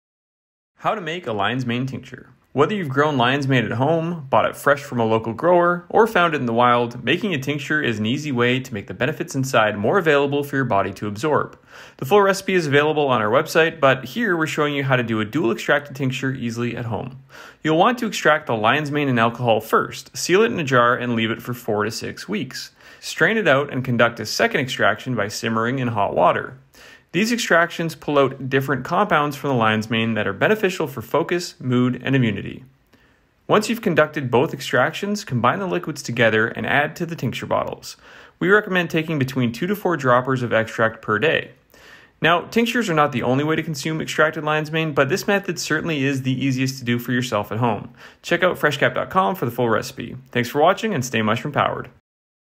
How to Make a Lion's Mane Tincture (super easy)
Want to make your own mushroom extract? Tinctures are an easy way to preserve your mushrooms and pull out those beneficial compounds for your body to absorb. Here we conducted a dual-extraction on some dried Lion’s Mane: a mushroom touted for supporting focus, mood and immunity. Extracting in alcohol AND hot water is necessary to pull out different compounds that are important for your body. Check out FreshCap.com for the full recipe and stay mushroom powered 🍄✌️